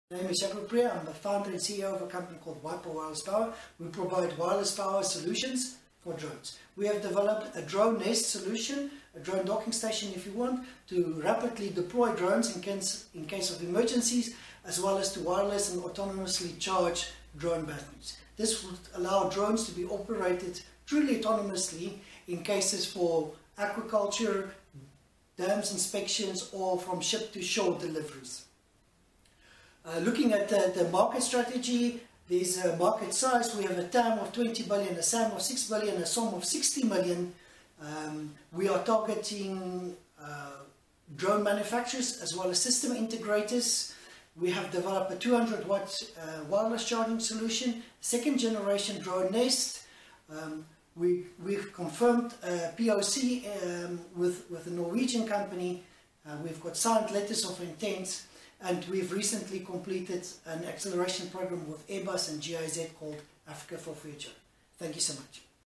WiPo: AHUB Pitch & Win 2019 shortlisted startup
WiPo have been shortlisted for the AHUB Pitch & Win 2019! If you think this startup could transform Africa, cast your vote for them